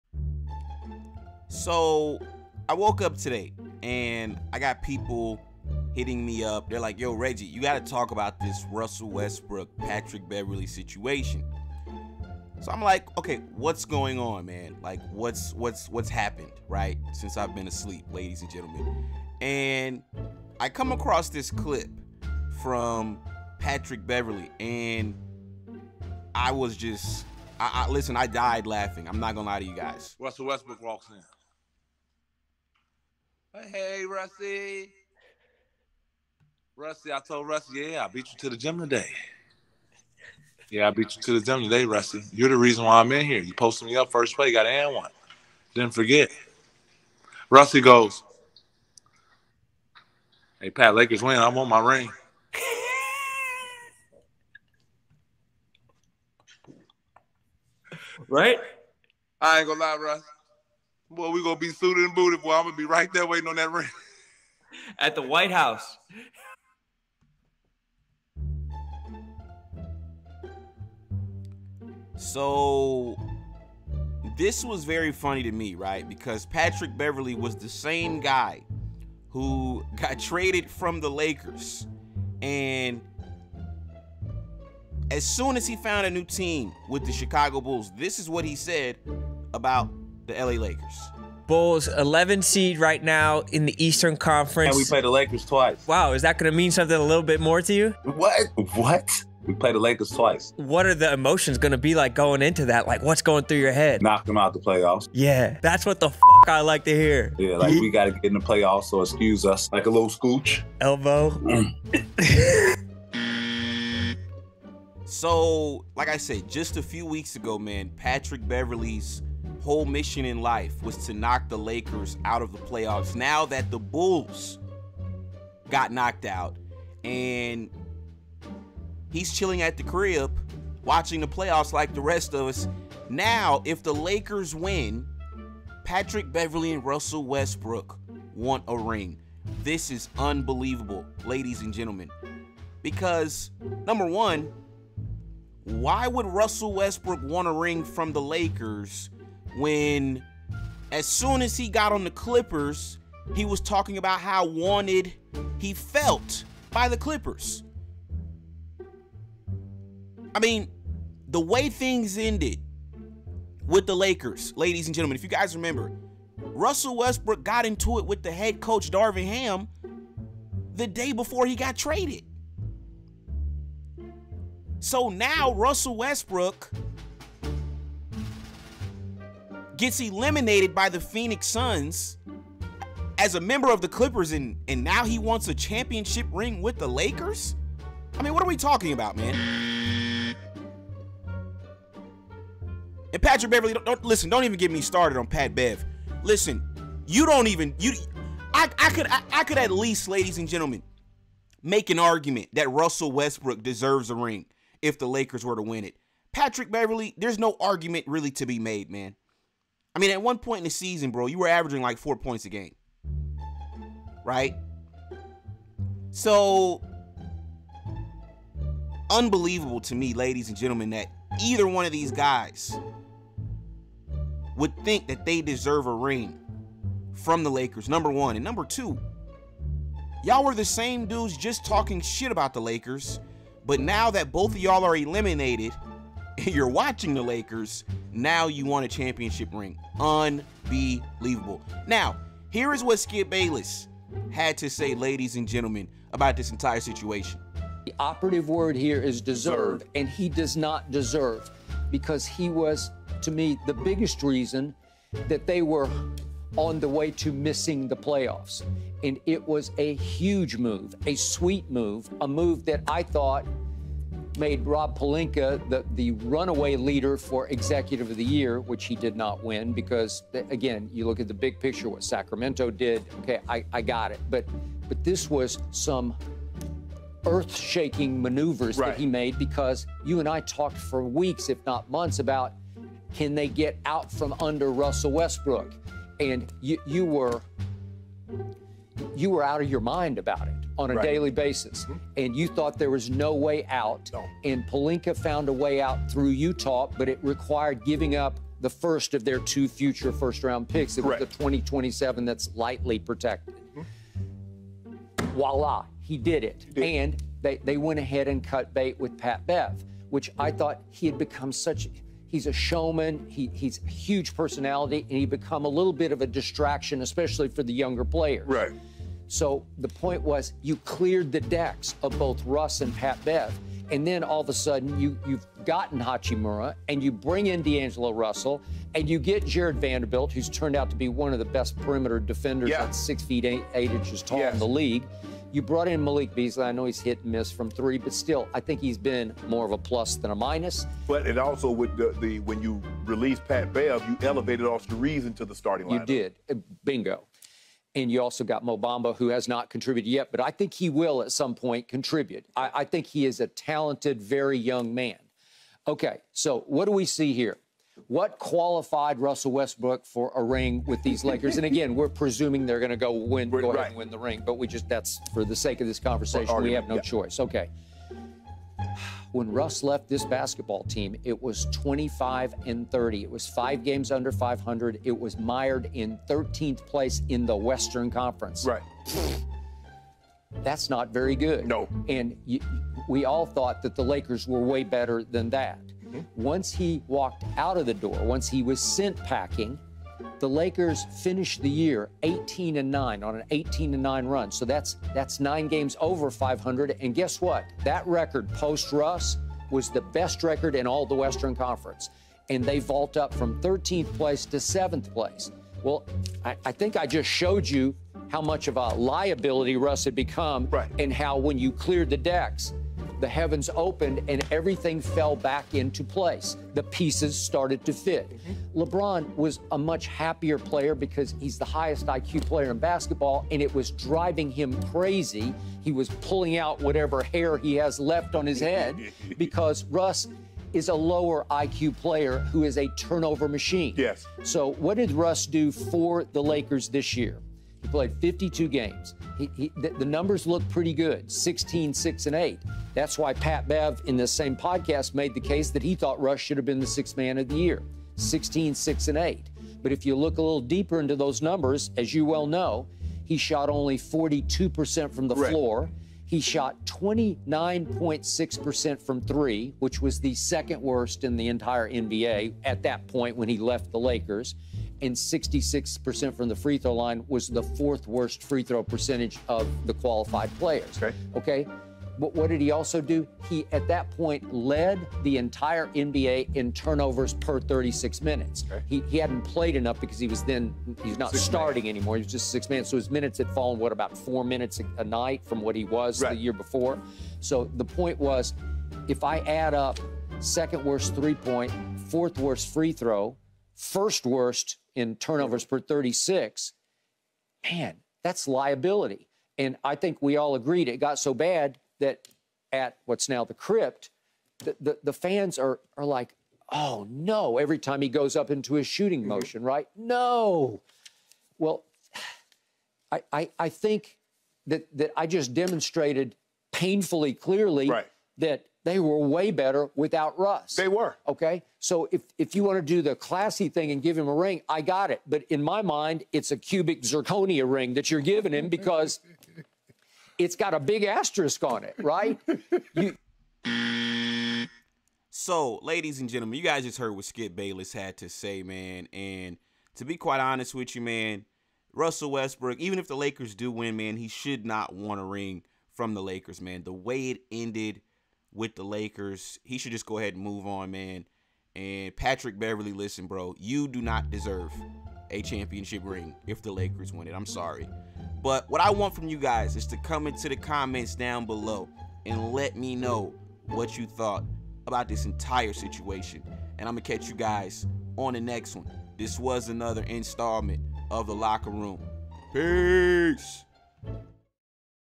You Won't Believe What These Former Lakers Said...
Russell Westbrook DEMANDS a Ring if Lakers Win and Skip Bayless RIPS him: 'HE DOESN'T DESERVE IT'

Russell Westbrook and Patrick Beverley each started the 2022-23 season on the Los Angeles Lakers, and they want their championship rings if the Purple and Gold take home the title in the playoffs.

During an appearance on the Pat Bev Podcast with Rone (10:18 mark), Beverley said he ran into his former teammate in the gym. That is when Westbrook told him, "Hey Pat, Lakers win I want my ring."

Beverley said he agreed with Westbrook and also wants a championship ring.

Westbrook played 52 games with the Lakers this season before they traded him to the Utah Jazz. He never played for Utah and joined the Los Angeles Clippers after agreeing to a buyout with the Jazz.
Russell Westbrook Throws It Down

Beverley played 45 games for the Lakers before they traded him to the Orlando Magic. Like Westbrook with Utah, he agreed to a buyout without playing for Orlando and then signed with the Chicago Bulls.

The moves were part of a dramatic roster shakeup for the Lakers that included bringing in D'Angelo Russell, Jarred Vanderbilt and Malik Beasley. The team has far more effective depth at this point and is starting to look like a championship contender with a 1-0 lead over the Golden State Warriors in the second round.

Los Angeles already eliminated the second-seeded Memphis Grizzlies in the first round.

It is quite the turnaround for the franchise after a 2-10 start that made even qualifying for the playoffs seem like a long shot. Part of the problem was Westbrook being a less-than-ideal fit alongside LeBron James and Anthony Davis because of his poor outside shooting and the need to have the ball in his hands to be an effective playmaker.

While the two guards were members of the Lakers for more than half the season, it would feel somewhat hollow if they each received their first championship ring in this fashion.

That is especially the case for Westbrook, who is an all-time great with a spot on the NBA's 75th Anniversary Team. Championships remain one way the best players in the sport's history are compared to each other, and Westbrook getting one for a team he didn't even suit up for in the playoffs wouldn't have the same meaning.

Still, he was part of the team for some time and clearly wants the ring more than he is concerned about any narrative-fueled pushback to receiving it.